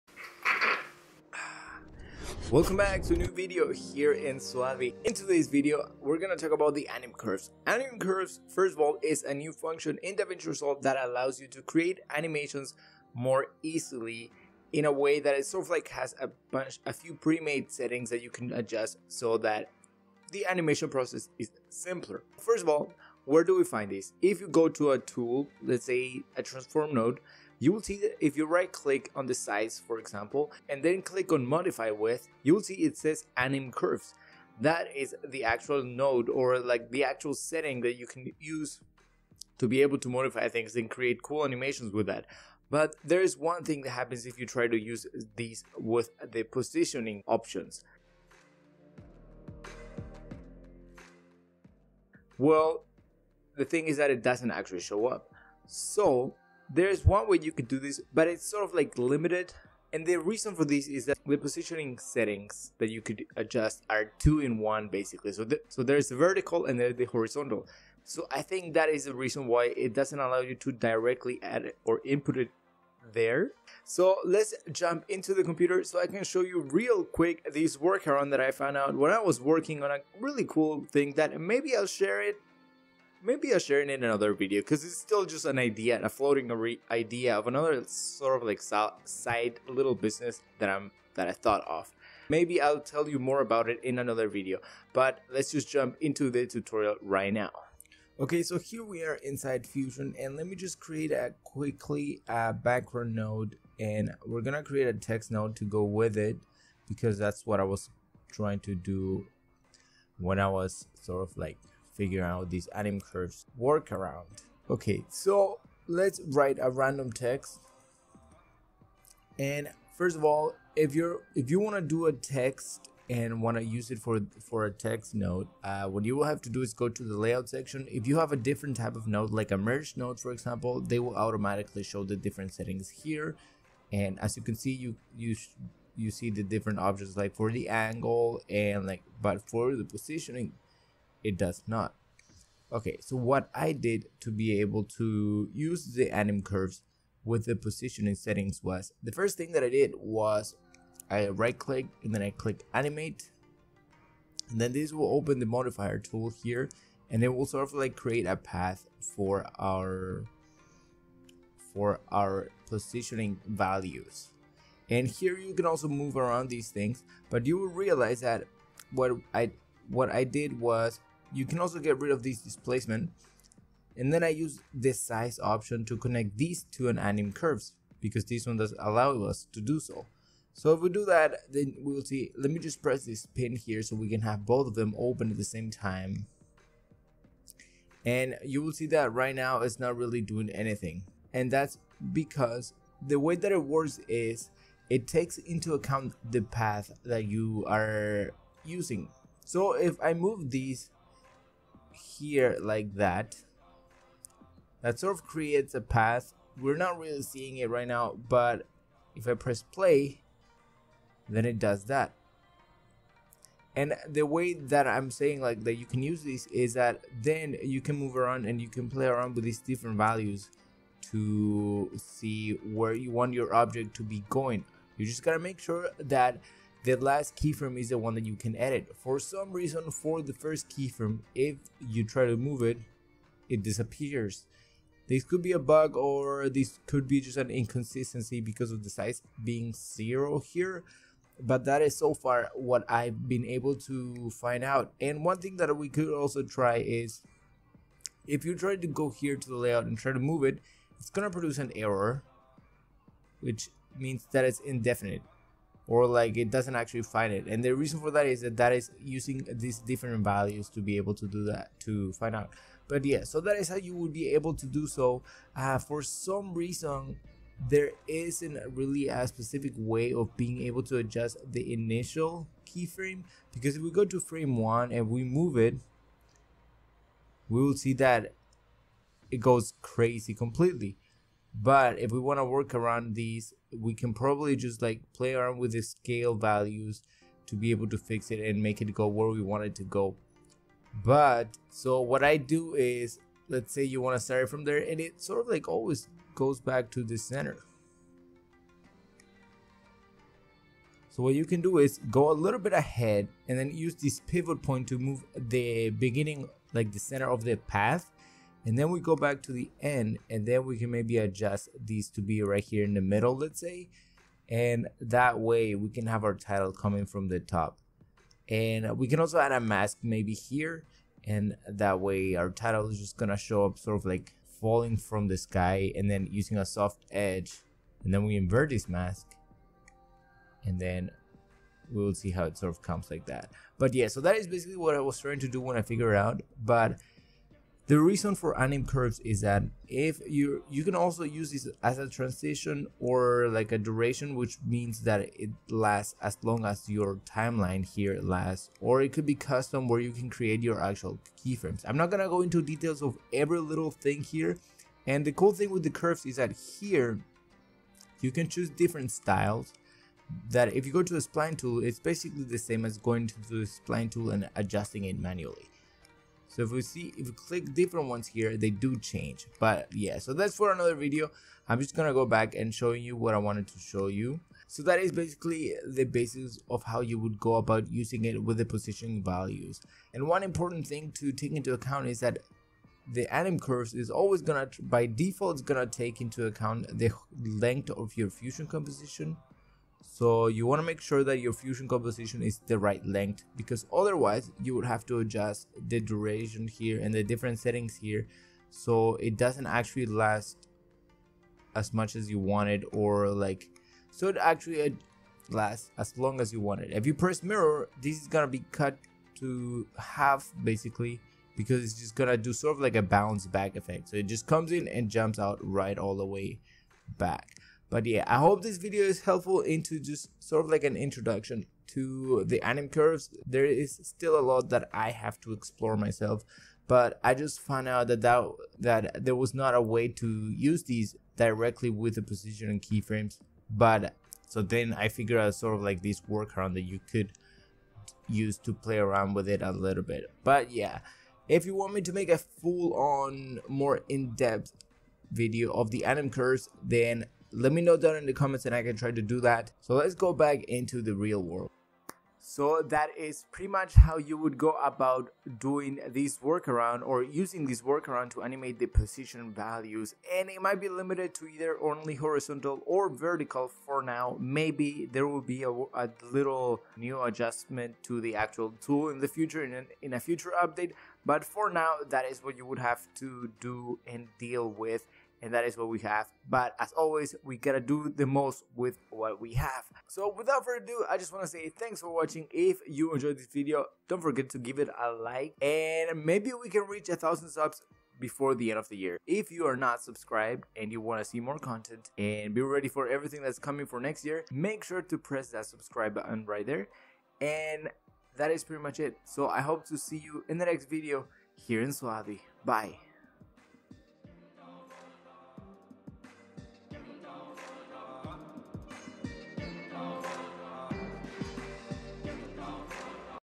USING Anim Curves In DaVinci Resolve
Hey guys in this video I am going to talk about using anim curves in DaVinci Resolve, but more specifically a WORKAROUND when trying to use them with  the Position Values of an element.

Why does this happen, well, I think it is because for the Positioning, there are 2 different values in the same place. The X and Y, and that messes up everything, or makes it harder to make the anim curves work easily, I am sure the developers are working already on a something about this.

But the good news is that I figured out a way that maybe you can use if you want to use the Anim Curves with the position values.

I hope this DaVinci Resolve tutorial is helpful, and I hope that you learn something new today.

I hope that the end of the year comes with good news and opportunities to look into next year.
Kev
SUALVI

I have over 100 videos that you can enjoy and plenty of free davinci resolve templates that you can download too .